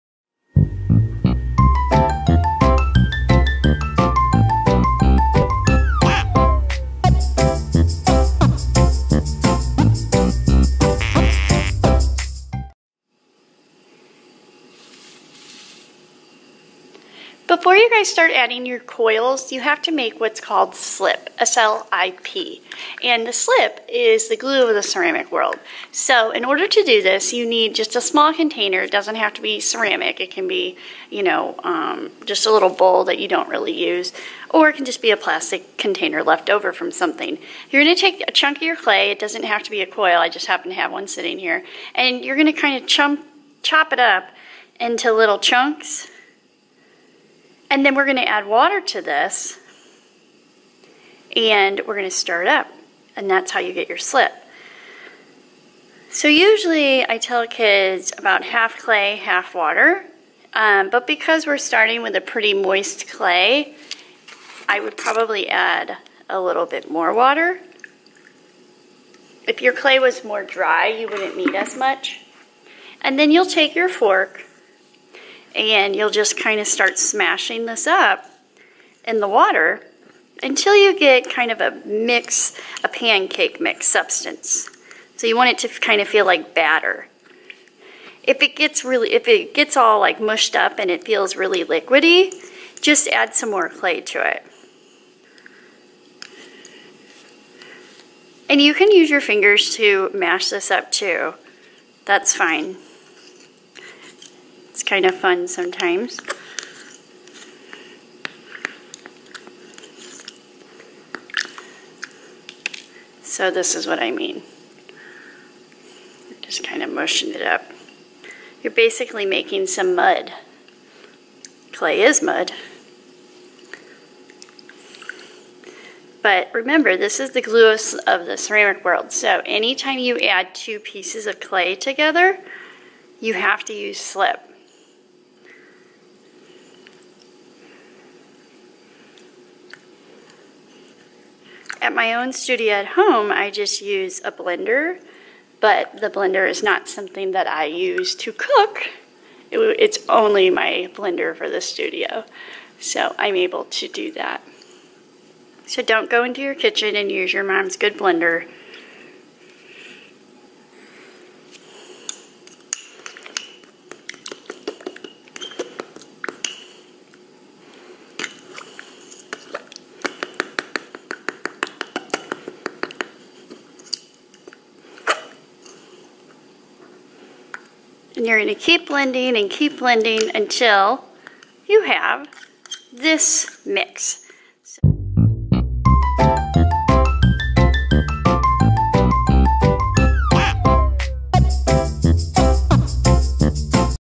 How to make slip for ceramics?
SLIP=Glue of the ceramic world. If you have an old blender use it to mix your slip. If not this video shows you how to make slip the old fashion way.